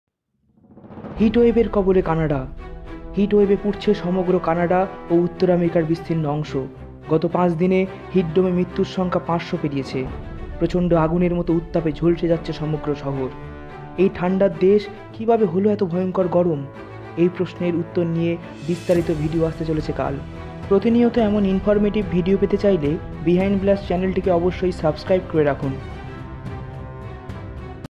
Canada hot Weather /Canada Temperature/Canada Heatwave deaths/ Heat Dome explained
Welcome to Behind Blast 🙏
This is a type introductory Video.
Tomorrow the Details video will come.
In that Video we are Discusse about Canada Heatwave Temperature or Canada hot Weather. In that Video we also Discuss about Canada Heatwave deaths and Canada Temperature. We Also Do Heat Dome explained.